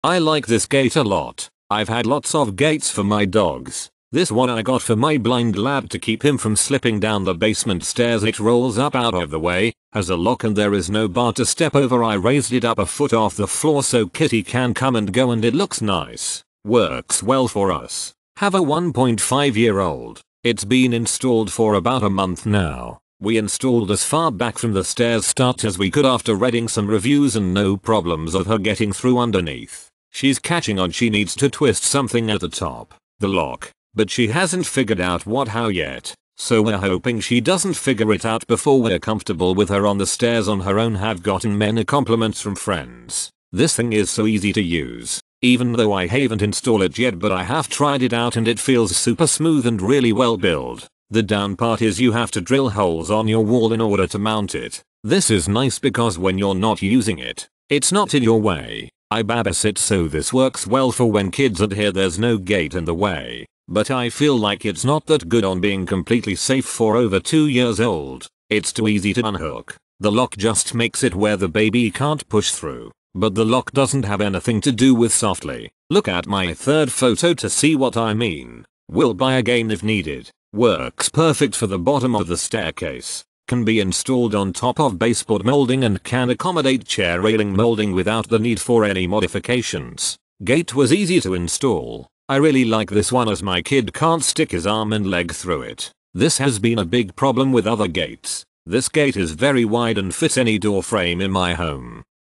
Retractable Baby Gate, Momcozy Mesh Safety Gate for Babies and Pets, Extra Wide review
Retractable Baby Gate, Momcozy Mesh Safety Gate for Babies and Pets, Extra Wide Safety Baby Gate 33.7" Tall, Extends to 55" Wide, Pet Dog Gate for doorways, Stairs, Hallways, Indoor//Outdoor (Grey)

Main Features:
- Retractable Design-Retractable baby gate extends up to 55" wide and is 33.7" tall, designed to adjust to any door/stair width up to 55 inches. Mesh can be completely retracted away when not in use, saving you space.
- Lock Feature-There is a locking mechanism at the top of the dog gate that turns to lock and unlock, when you're busy something, effectively blocking the baby and small & medium dogs to ensuring them safety, providing a safe and closed off environment to play.
- Moveable-Comes with 2 sets of Installation hardware, pet gate can be easily moved and de-attached from the gate mount hardware, to stow away or move to another location where another set of hardware installed. Can be used indoor and outdoor.
- One Handed Operation-Just gently press and rotate the ball cap to lock and unlock baby gates with one hand and can easily pass through. All make it super convenient to use when you're carrying toys, diapers, or little kiddos around.
- 2 Years Quality Assurance-We provide lifelong technical consultation and customer service. Just contact Momcozy customer service if any issues happened